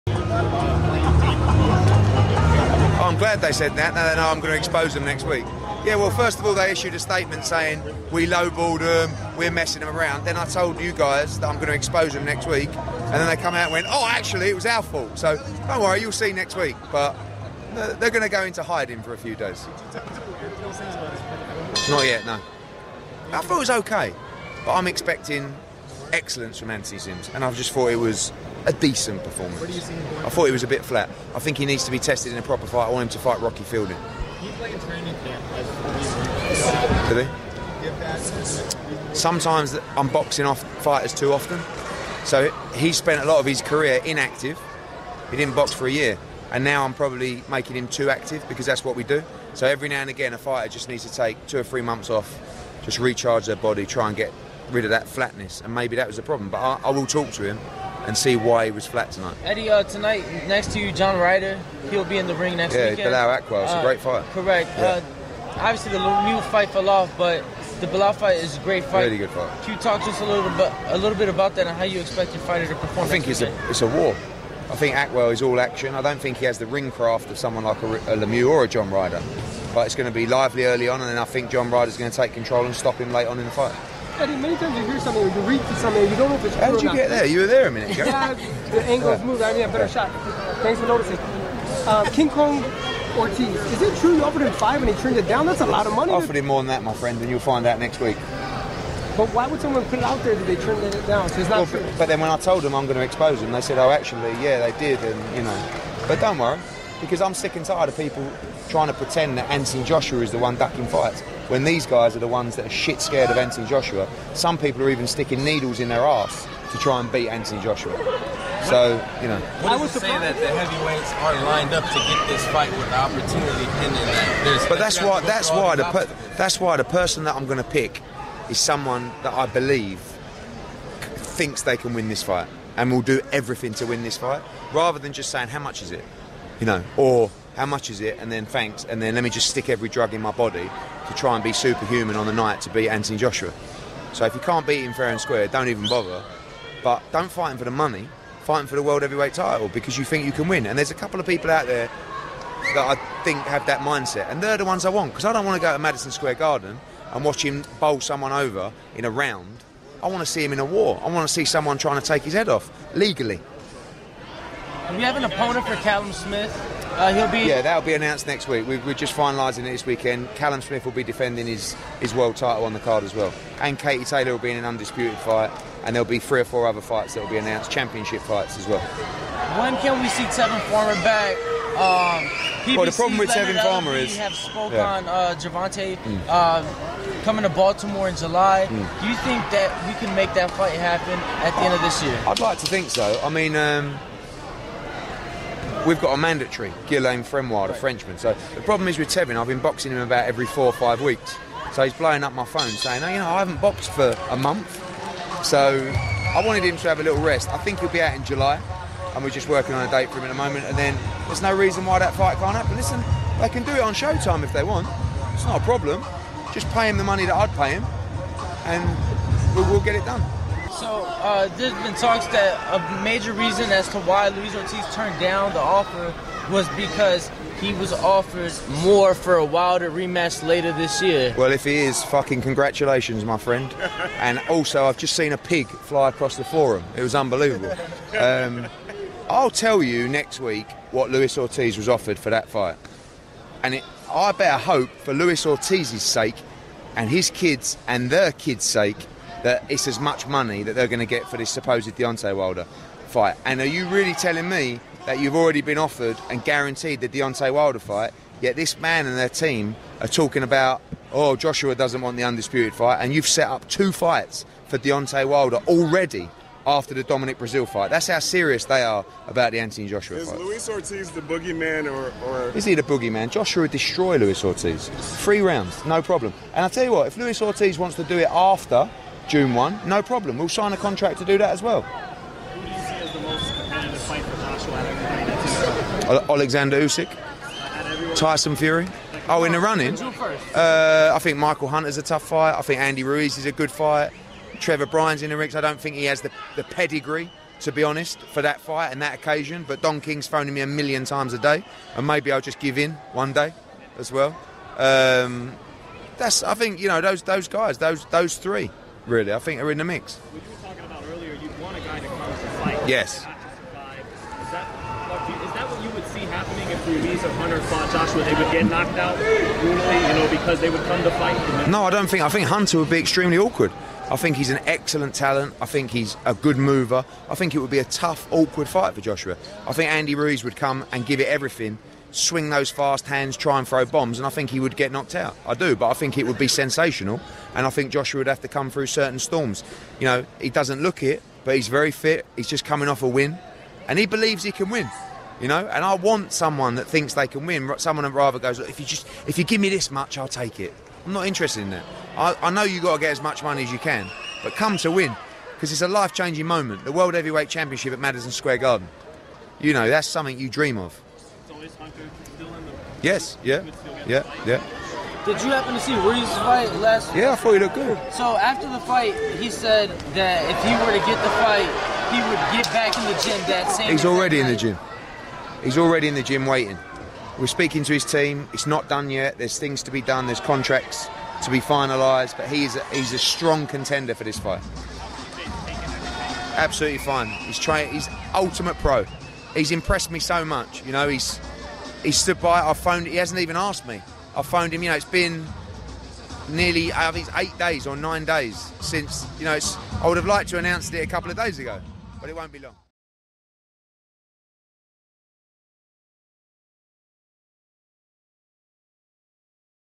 (WOW!) EDDIE HEARN GOES OFF ON LUIS ORTIZ & "SH*T SCARED" JOSHUA CHALLENGERS; JUNE 1 OPPONENT UPDATE
FightHype.com was on hand at the Forum in Inglewood, California where Juan Francisco Estrada scored a unanimous decision victory over Srisaket Sor Rungvisai to capture the WBC super flyweight title. You don't want to miss what those in attendance had to say about the performance and much more. Check it out!